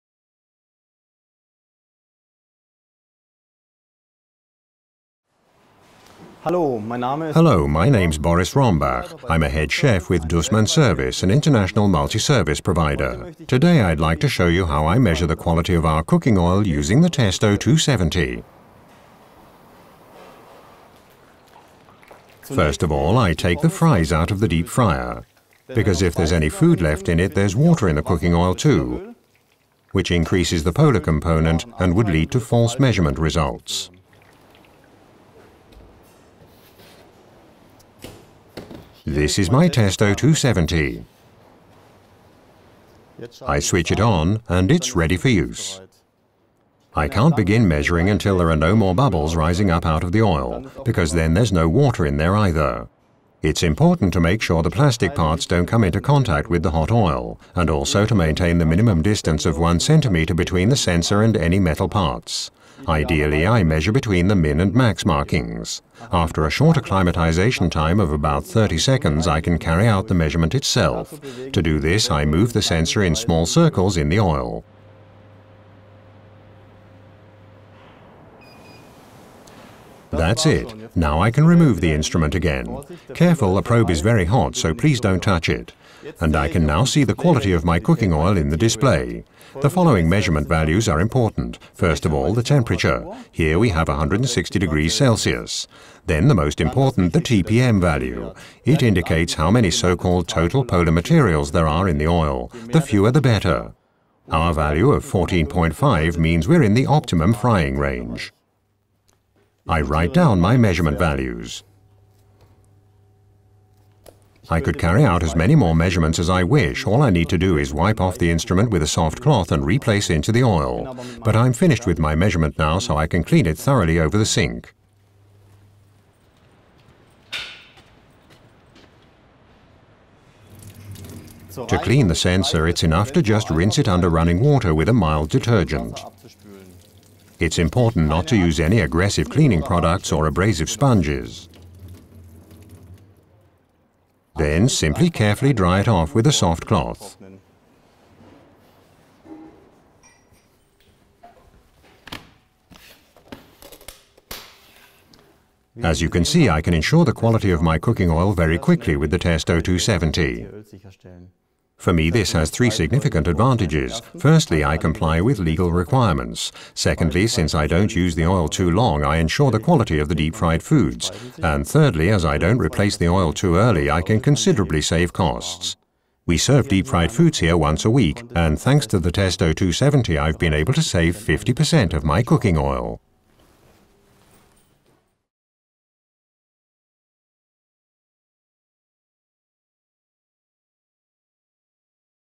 Measure the quality of cooking oil - Testo 270 Oil Tester
The testo 270 allows you to accurately determine the optimum time to change cooking oil, by measuring the Total Polar Materials TPM) of the cooking oil; these are the chemical products that emerge during the frying process such as free fatty acids and mono/di-glycerides.  Measurements can be carried out directly in hot oil and there is no need for the sensor to cool between measurement, thus back to back measurements are possible.

Cooking oil that has been used for too long can affect the taste and quality of fried products and can lead to complaints from the customer .The testo 270 will ensure that oil use is optimised reducing costs and ensuring consistent quality of the fried product.

    Measurement directly in the deep-fryer
    Sensor is embedded in metal and breakage-proof, can be easily wiped clean thanks to sensor protection layer
    Fast measurement value display in % TPM
    Automatic recognition of measurement end (Auto-Hold %TPM)
    With 2 freely selectable limit values
    Alarm function aubible and visual:
    - Lettering "ALARM"
    - 3-colour LED bar (green, yellow, red)
    Configuration menu and limit values can be locked to protect them from external manipulation
    Removable protective case and optional hand strap (dishwasher-proof)
    Conforms to: VO (EG) 1935/2004, EC guideline 2004/108/EC